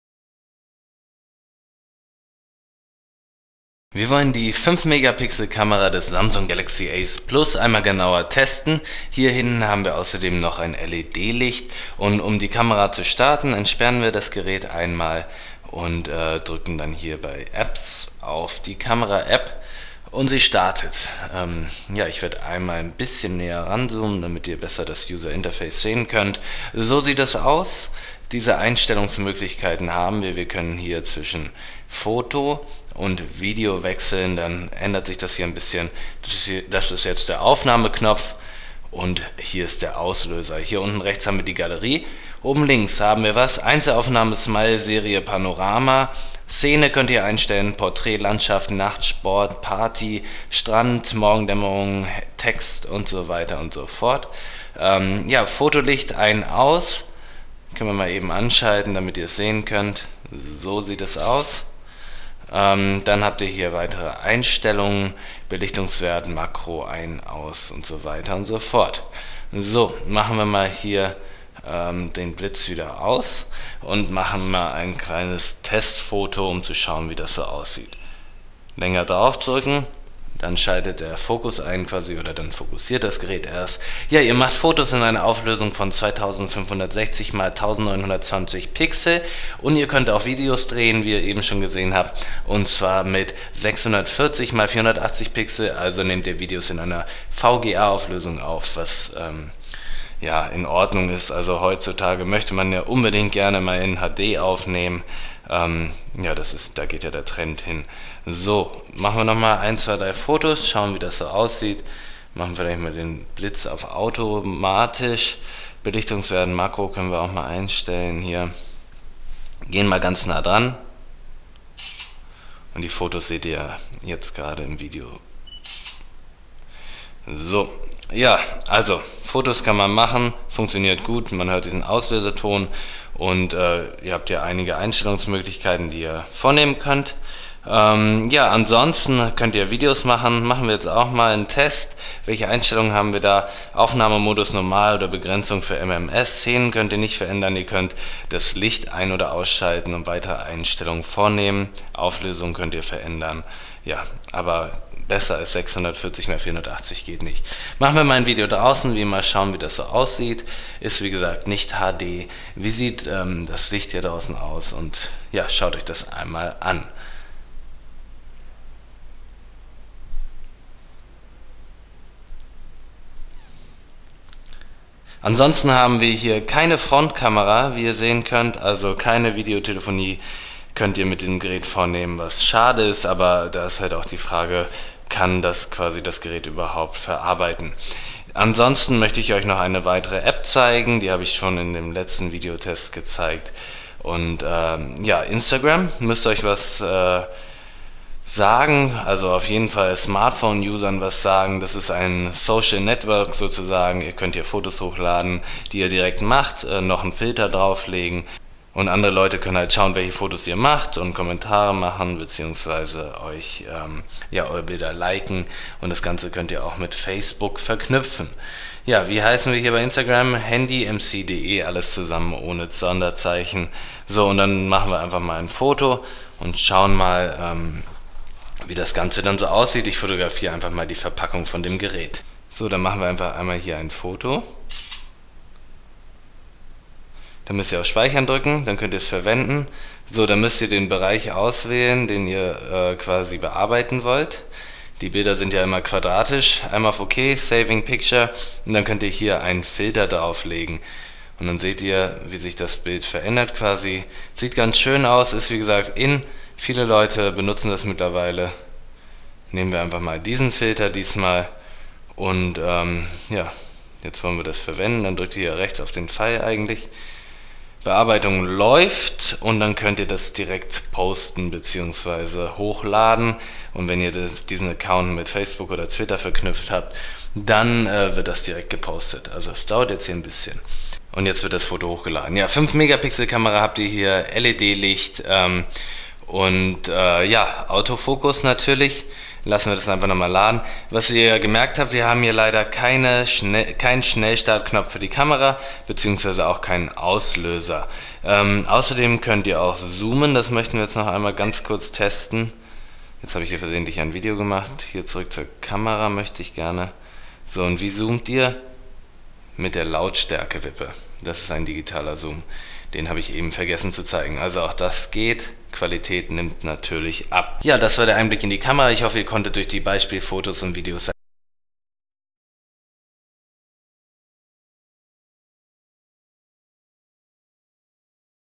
Samsung S7500 Galaxy Ace Plus - Kamera - Teil 4
Samsung S7500 Galaxy Ace Plus

Die Redaktion von handy-mc.de testet die Kamera des Samsung S7500 Galaxy Ace Plus. Weitere Informationen zu diesem Handy sowie Meinungen, Erfahrungsberichte und Fotos findet ihr